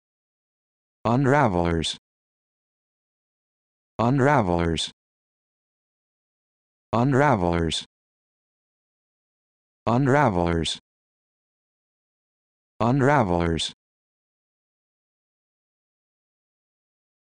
How to Pronounce Unravelers
This video shows you how to pronounce Unravelers